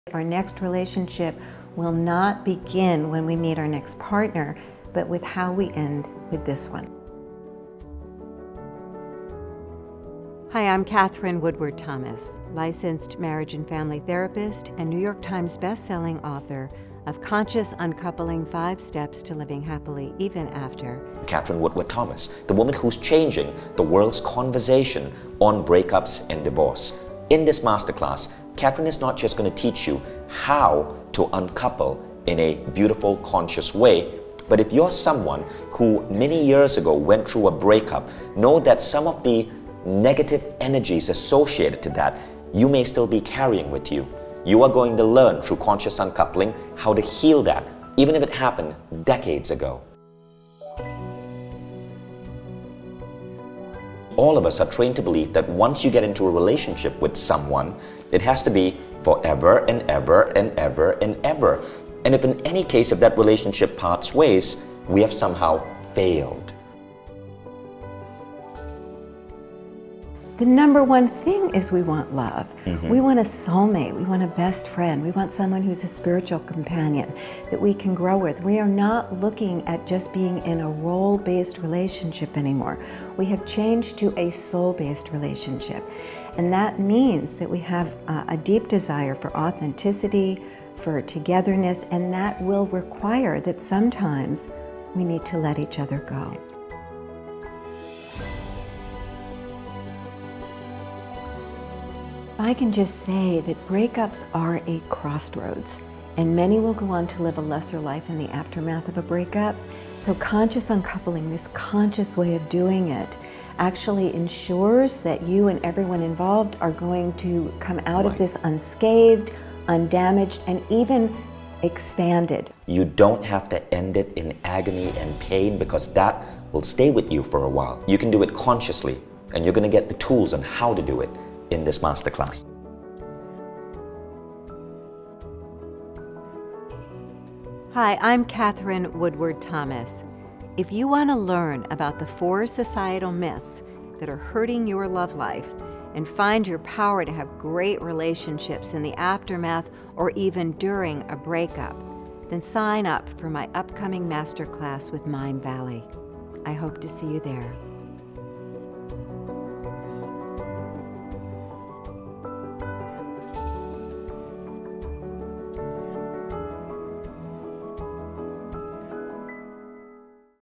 How To Heal From A Breakup With Katherine Woodward Thomas | Mindvalley Trailer
A bad break up can be one of the most traumatic experiences most of us will go through 💔 But there’s a simple 5-step process to help you break free from the pain of your uncoupling and ensure you never have to suffer it again

It’s called ‘Conscious Uncoupling’, brought to us by best-selling author and relationship expert Katherine Woodward Thomas.

So often, we leave our healing to time…but time doesn't heal all wounds ⏰

It can take years before we feel whole again. And even then, the hurt can still linger beneath the surface 🤕

Conscious Uncoupling is designed to help you heal the wounds of your heart and finally free yourself from the pain, overwhelm and grief of your breakup.

So if you’ve ever been totally cut up and hurt by a break up or never want to be again, this is the Masterclass for you.